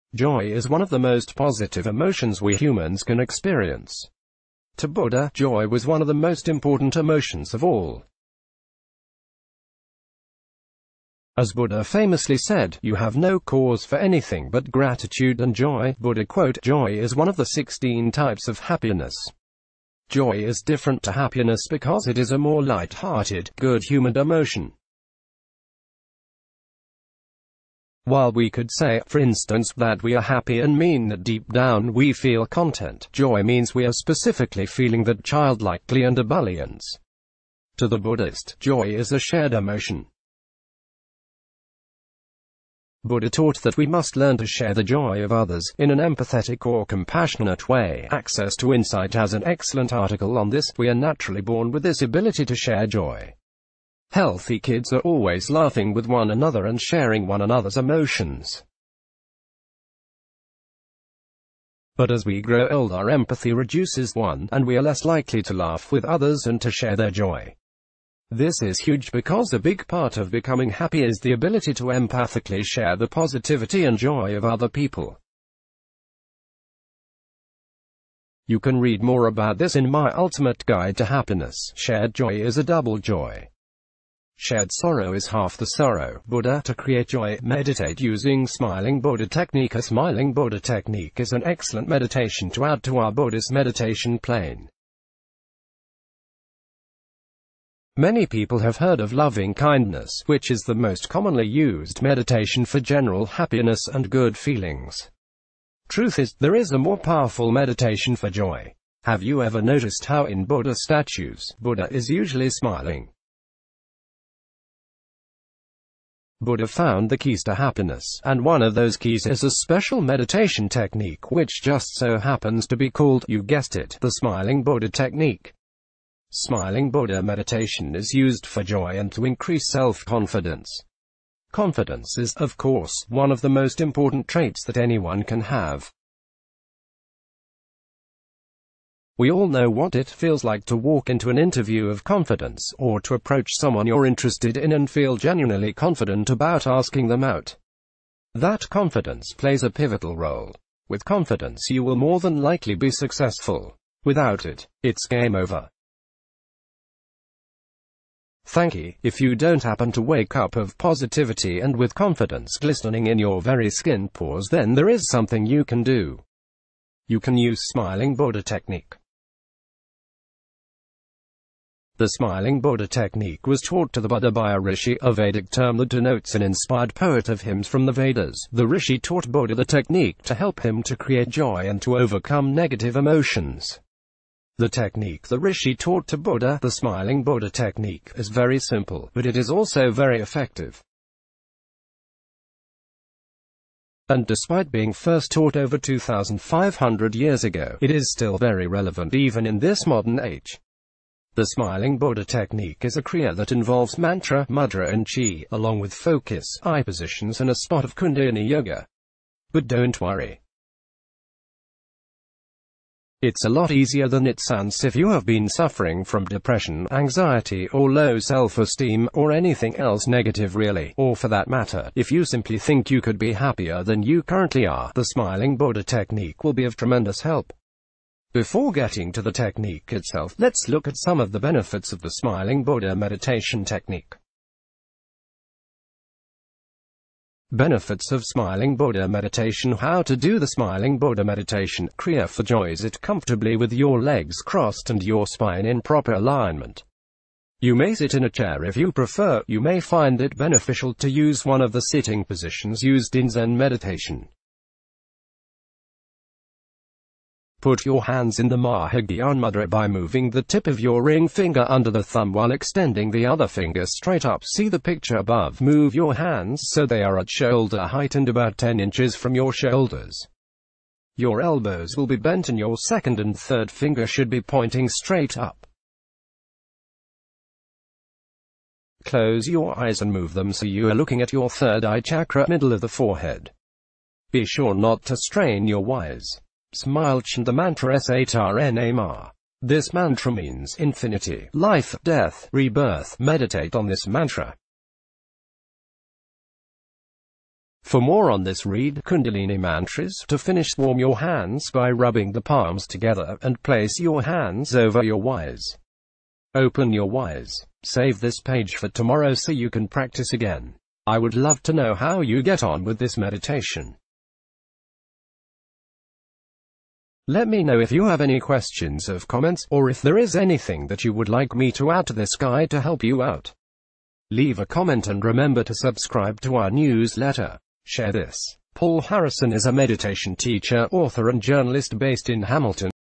How To Do Smiling Buddha Meditation To Create Joy | BuzzFresh News
Joy is one of the most positive emotions we humans can experience. To Buddha, joy was one of the mos...